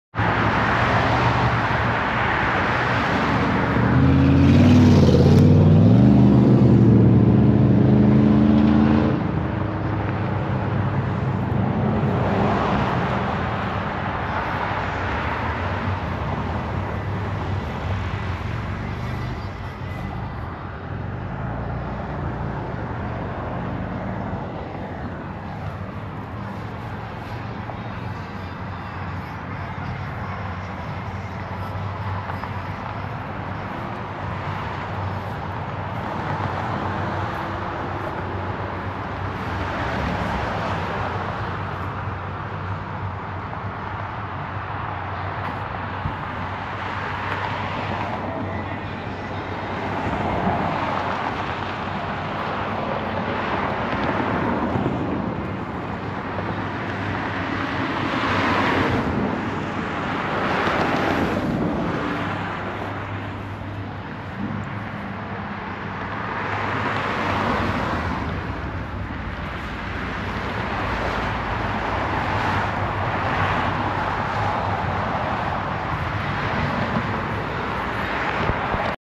1970 Dodge Charger and 1970 Dodge Coronet
Many people say 1970 was the height of the muscle car era. What do you think? Was it 1969 instead? The big three were cranking out some of the most iconic muscle and classic machines ever produced. Here are a couple muscle cars from the metro Detroit area. The 1970 Dodge Charger is owned by my friend who has had the car since 1979 and drove it in high school. Of course it has had a restoration, but it looks so cool to see it cruising down the road. The 1970 Dodge Coronet was made my way and gets driven often.